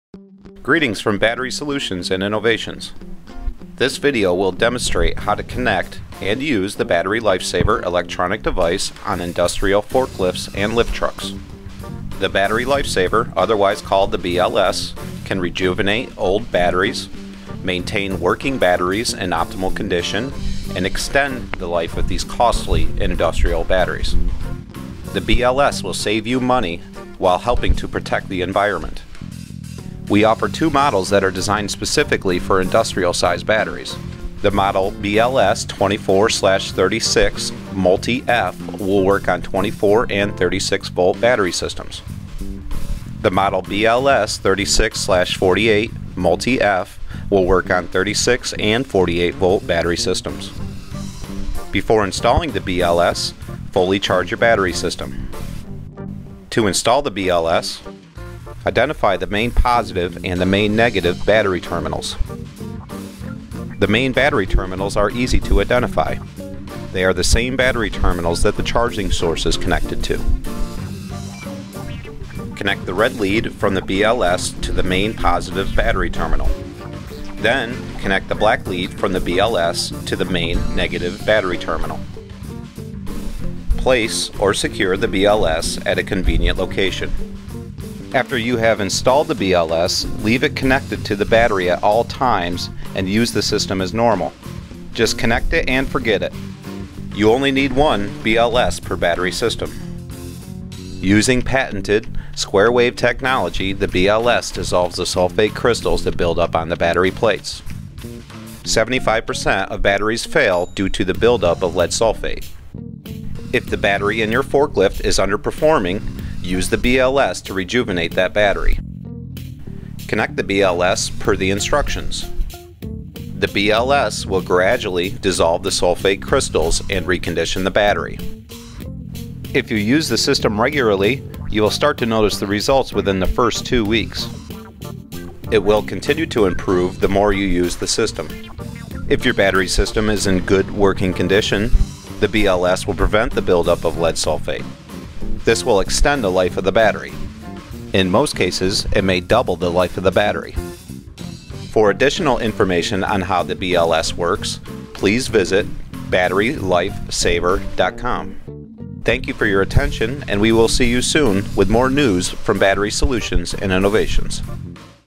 The BLS for Forklift Batteries
Rejuvenate your old forklift, lift truck and pallet jack batteries. The Battery Life Saver electronic device, the most effective desulfator available. Using patented square wave technology, the BLS rejuvenates old batteries, maintains working batteries in optimal condition, and extends battery life. The BLS will work on any lead-acid battery. We offer different models depending on the voltage of the battery system (from 12 to 156 volts). Only one BLS is needed per battery system.